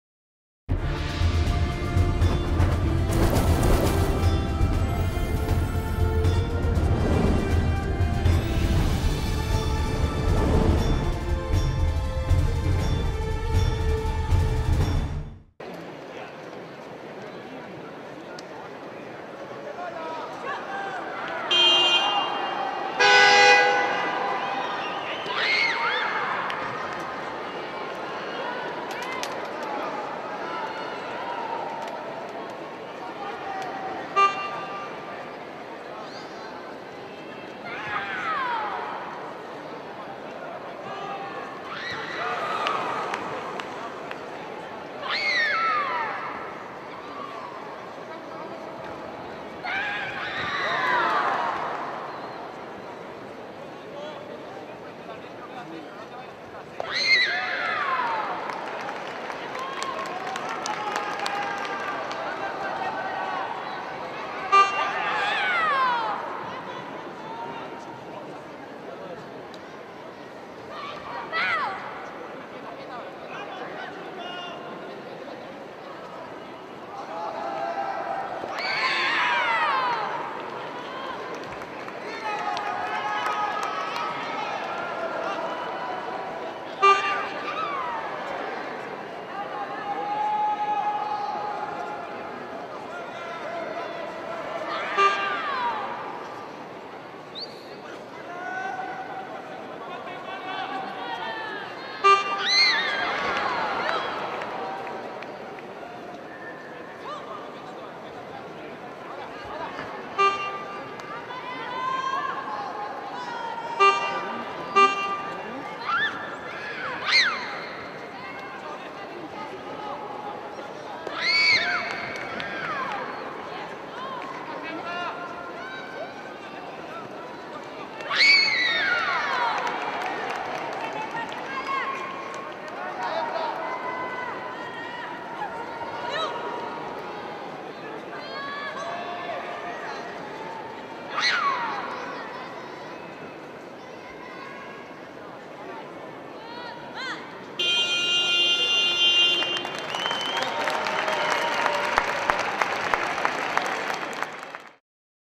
Sujeong Lim VS Euda Carias FINAL ROUND 1
WORLD TAEKWONDO TOUR MEXICO 2009
LIGA PROFESIONAL DE TAEKWONDO
First taekwondo professional fight

Trophy and price

Sujeong Lim winner against Euda Carias